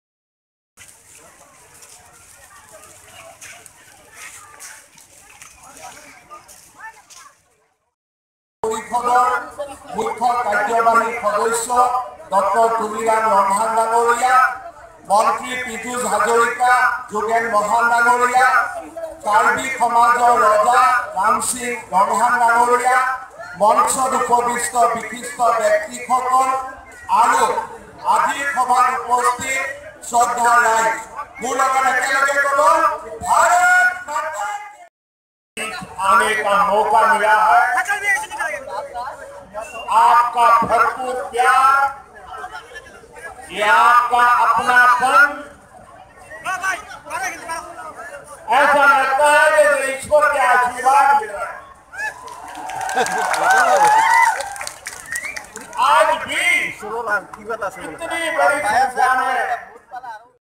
Assam Karbi Anglong Loringthepi Sjt.Narendra_Modi PM Of India Meeting Date 28 April,2022
Assam Karbi_Anglong_Loringthepi Sjt.Narendra_Modi Prime Minister Of India Assam Chief Minister Himanta_Biswa_Sarma Enjoy Meeting 28 April, 2022